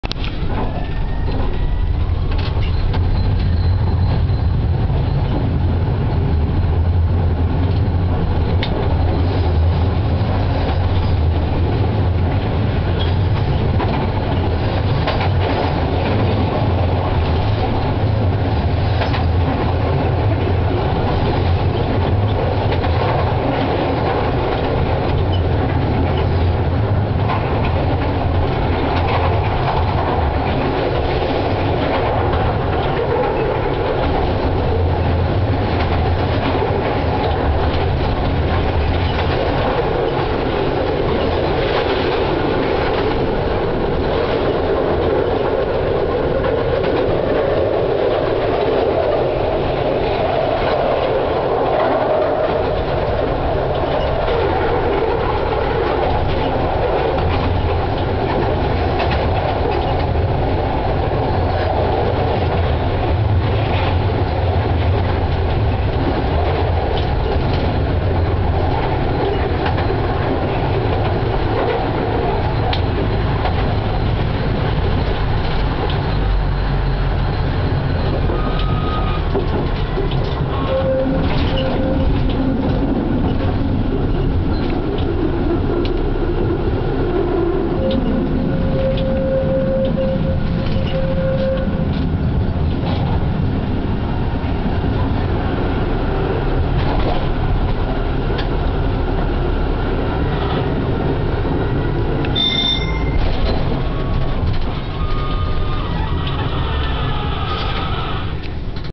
Tramwaje w Gdańsku - Stogi Plaża - Pasanil
Filmik kręcony w kabinie z "przelotu" ul. Nowotną ;)